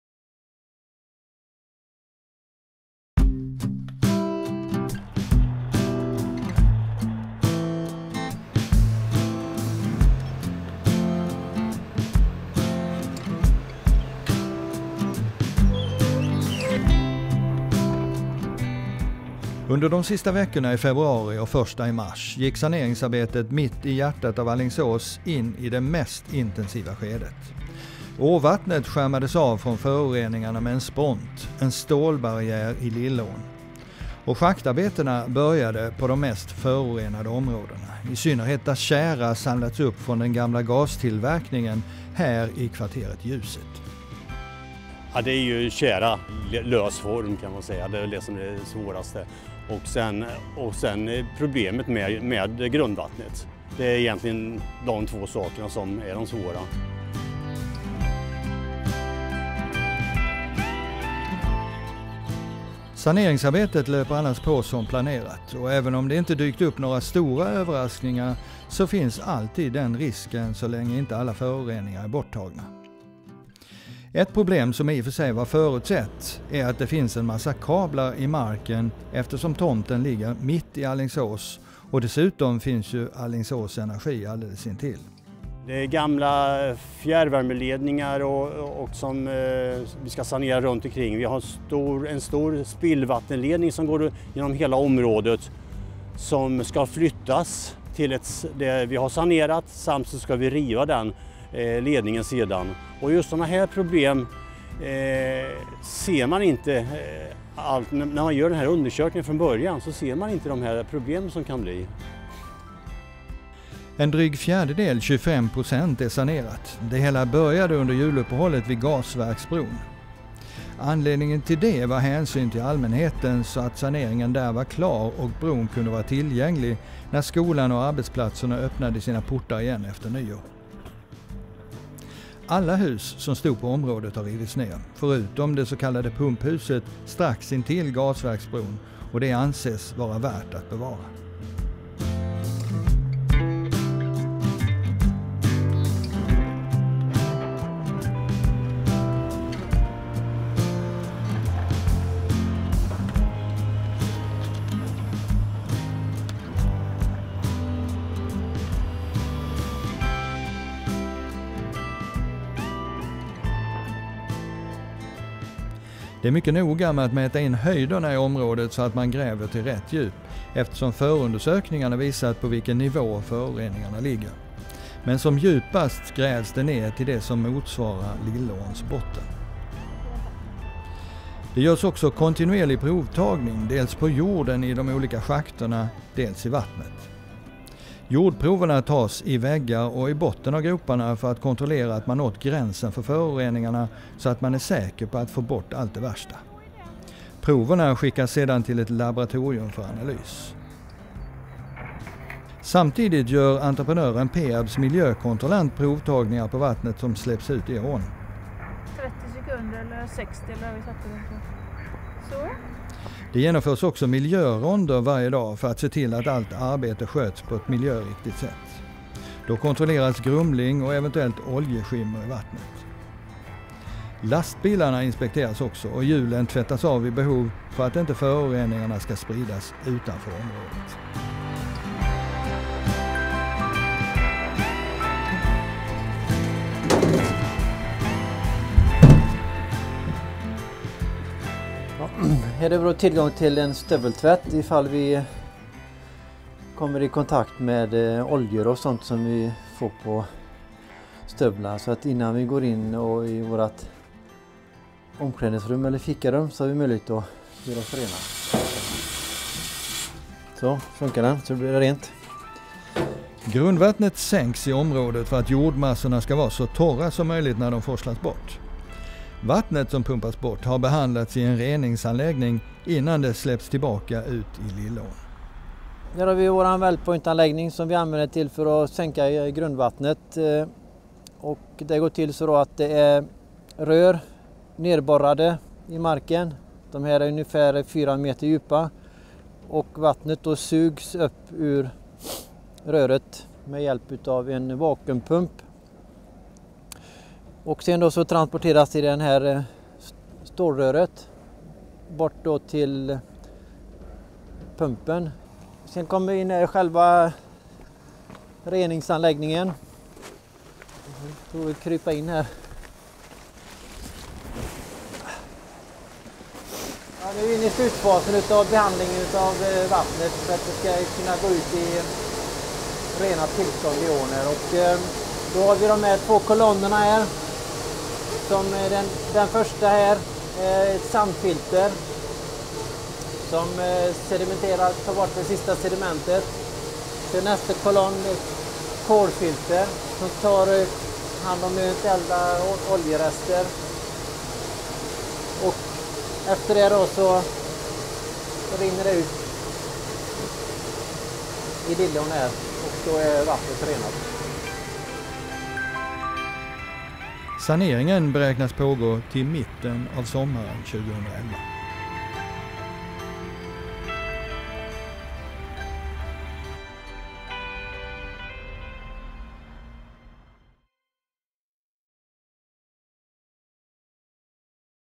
Alingsås kommun, sanering, xray film & tv
Saneringen av kvarteret Ljuset (Alingsås gasverk). Åtgärderna bedöms innebära att så gott som alla föroreningar inom Kv. Ljuset kommer att åtgärdas och området kommer att kunna användas enligt gällande detaljplan. Detta innebär att en del av Kv. Ljuset kommer att bebyggas med bostäder.